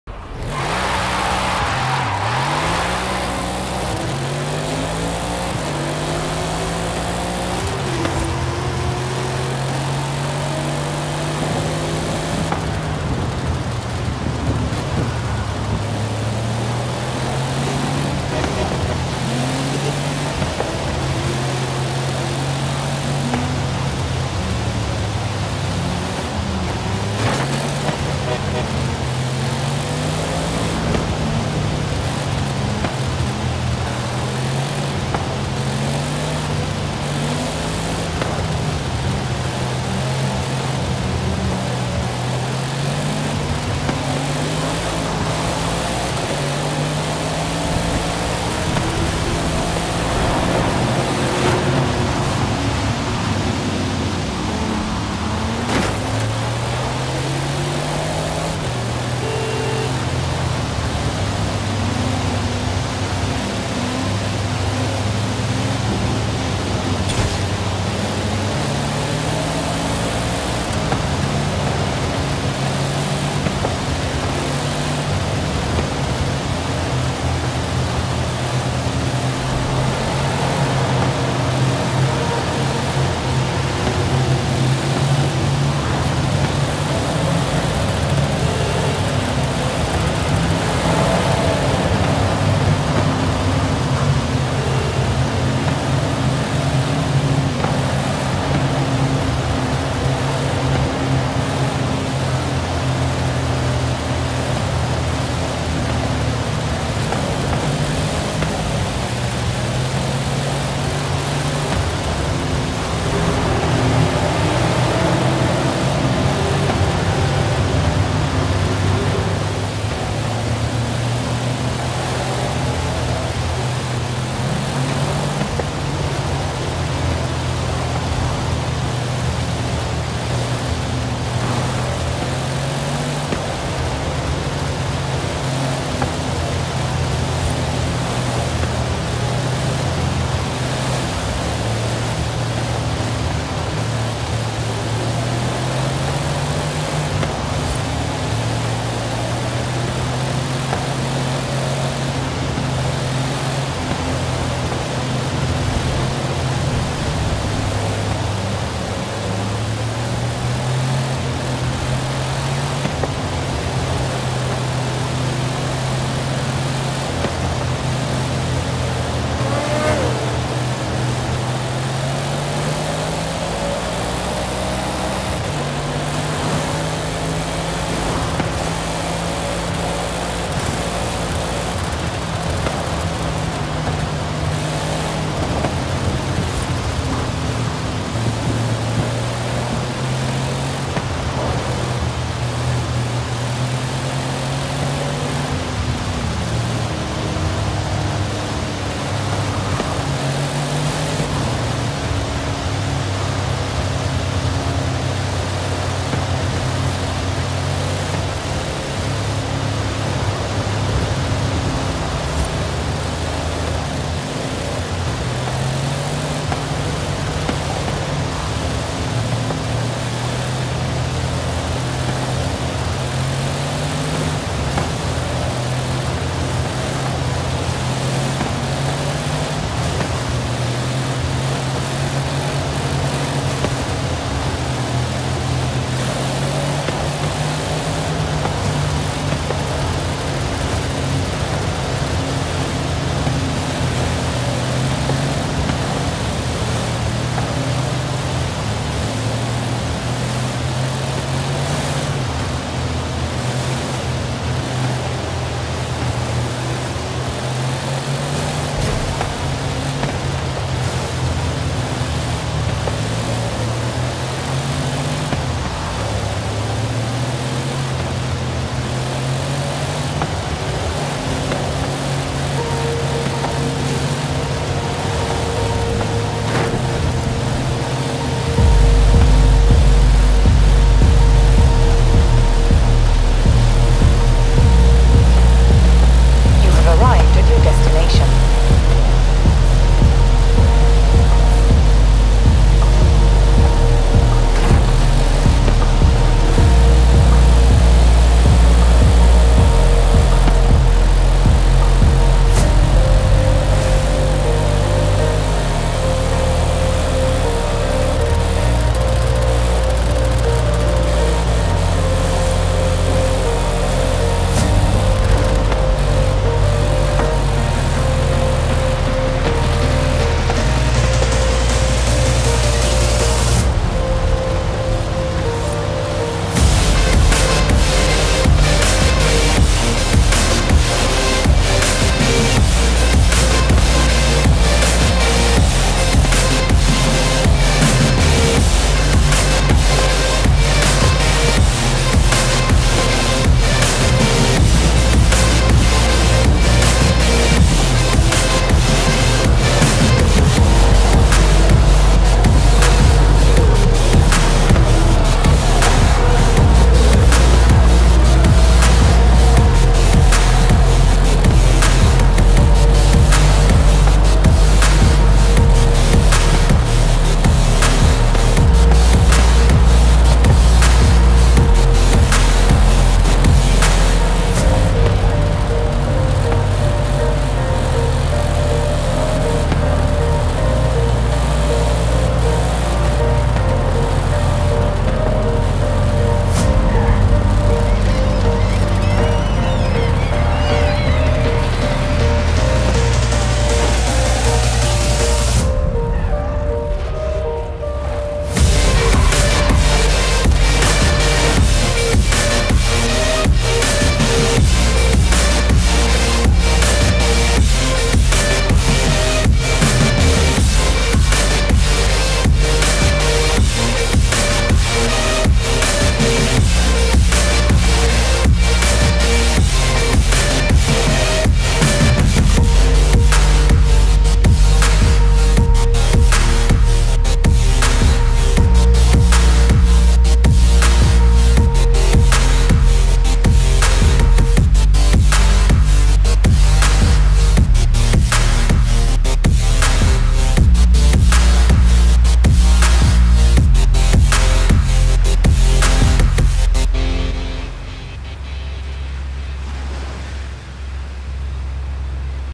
Audi RS7 2021 750HP GO CRAZY AT CAR MEET
Car: Audi RS 7 2021
Tuned: Quick lil Base setup
Tire: stock tire compound
750HP